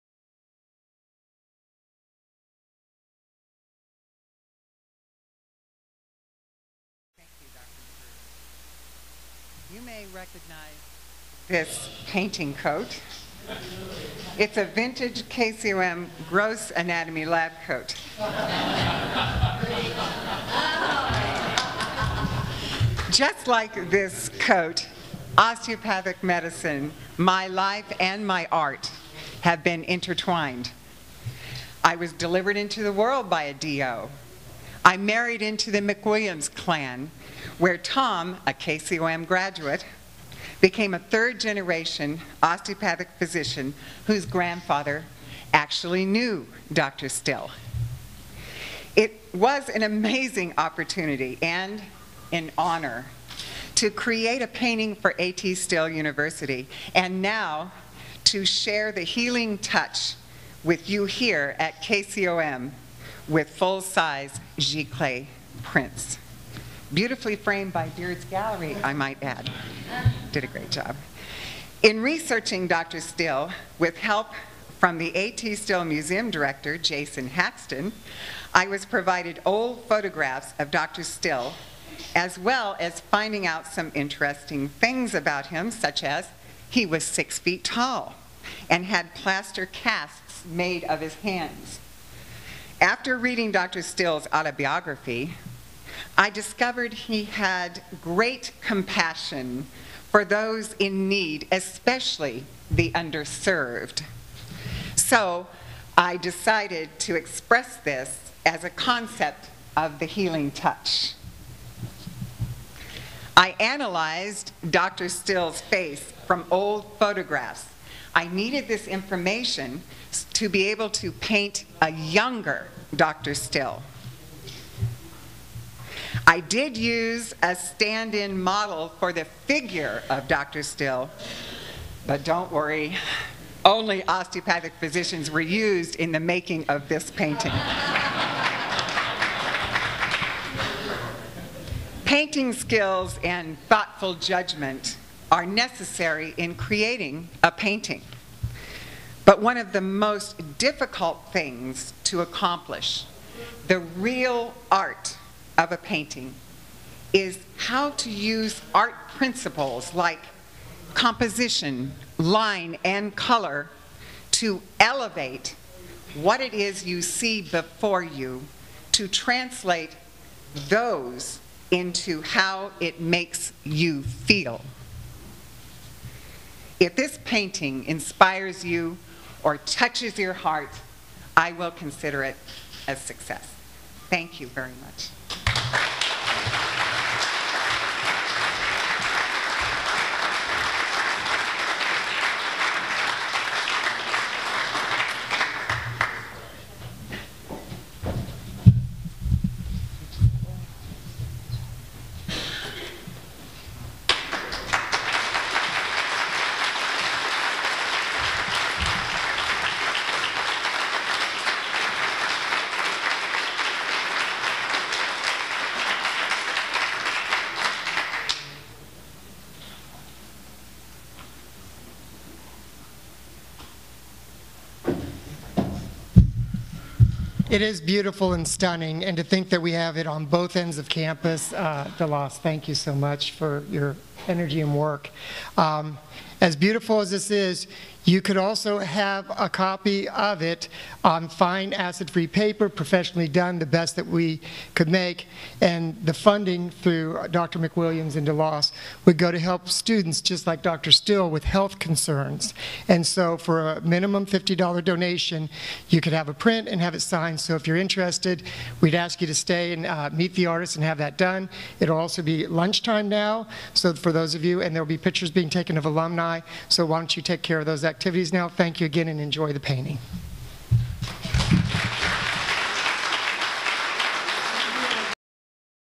Healing Touch Unveiling - ATSU Founder's Day 2010
Painter Patricia DeLoss McWilliams was commissioned to create this painting for ATSU, which is meant to inspire and reaffirm the importance to all health professionals of the sacred relationship shared with patients and their families.

To purchase a signed gilcée print and assist ATSU students and their families experiencing a health crisis, contact or . Prints are $50.